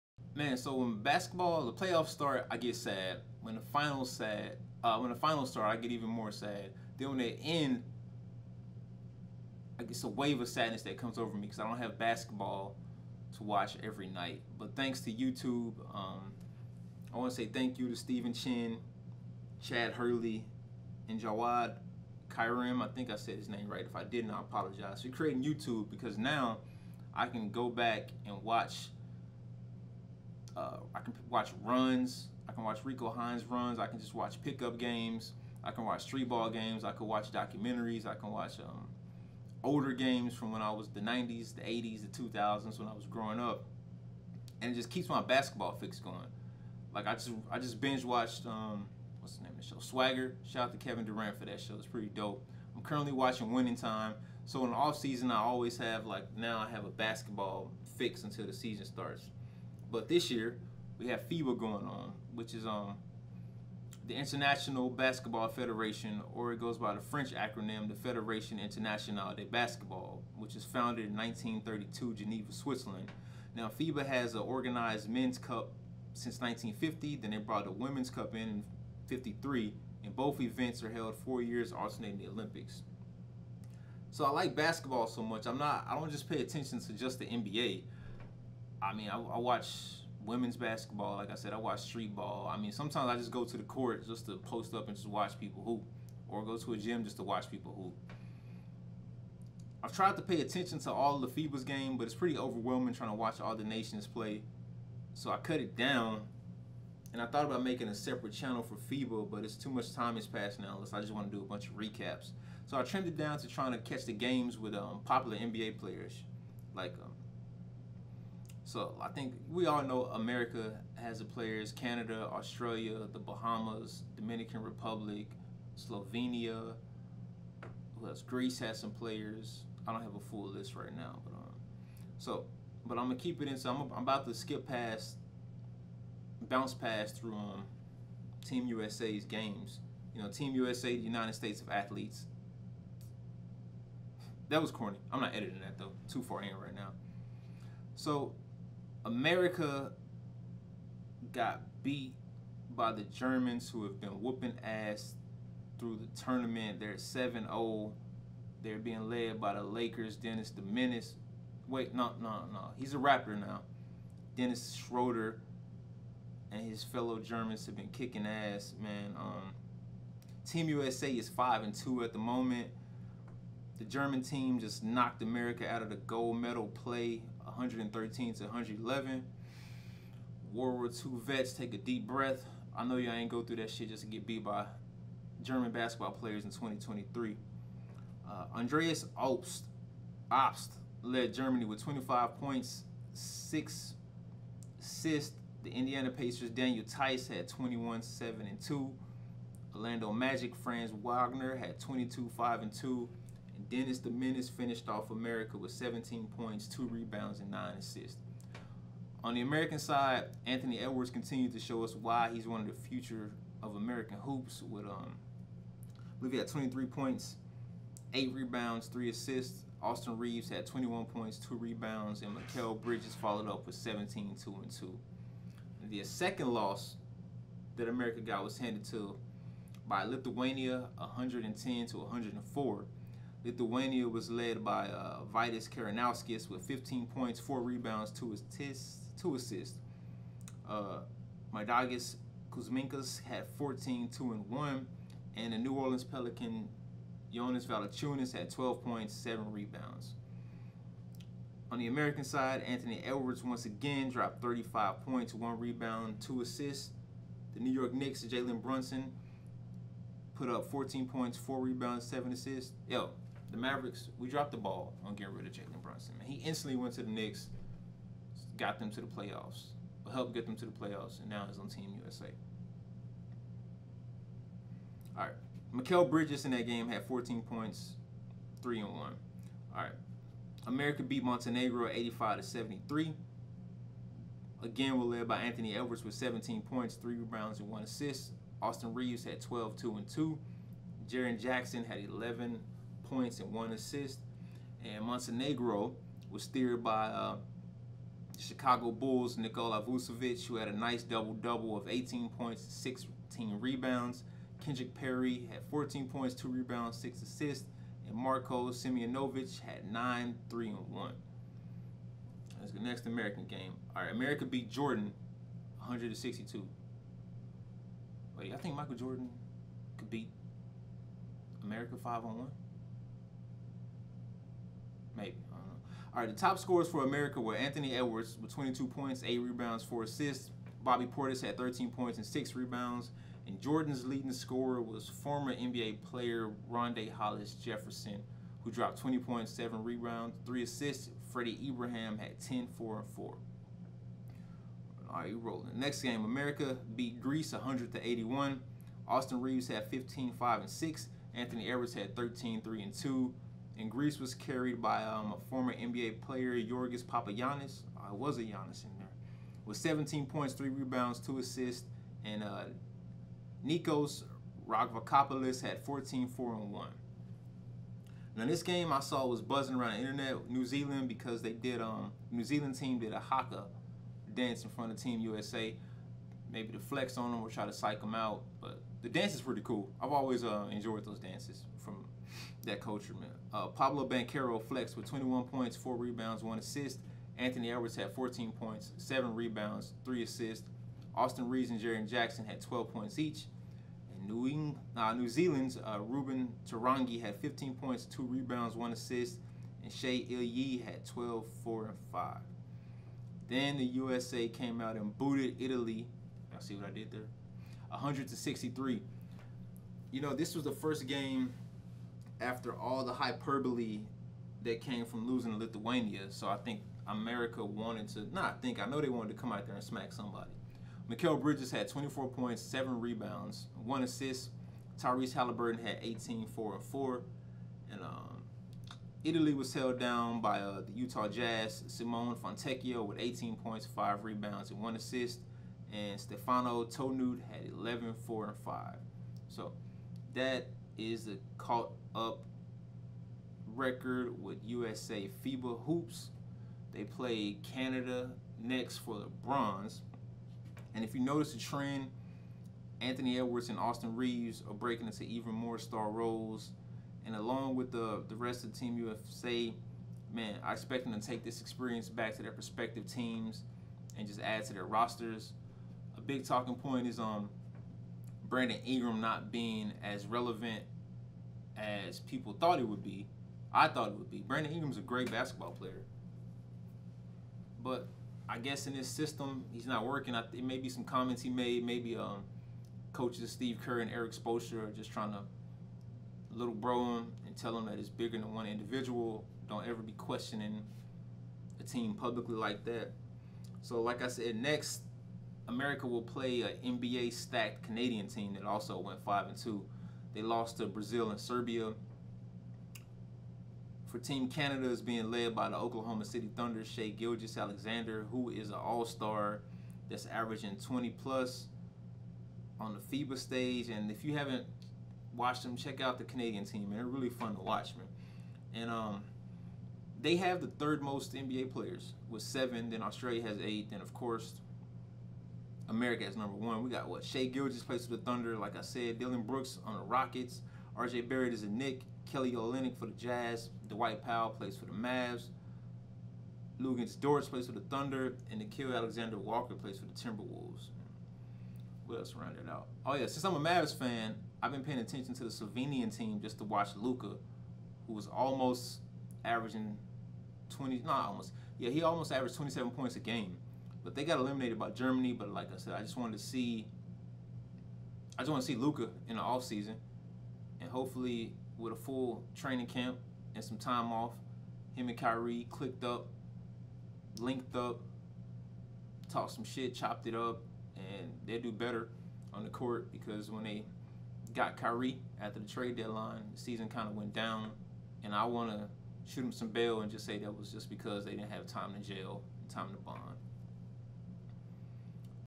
Honorable Discharge--FIBA Breakdown Pt. 1(Team USA and others)
Part 1 of me Euro-Stepping through Team USA Fiba journey.
Part 2 will cover the medal games. germany vs serbia, canada vs

1:00-Fiba Intro
2:30-USA vs Germany
3:46-USA vs Lithuania
4:41-USA vs Montenegro
5:14-USA vs Jordan
5:45-USA vs Greece
6:10-USA vs New Zealand
6:44-USA vs Italy
8:16-Canadian Team Intro
9:00-Luka talk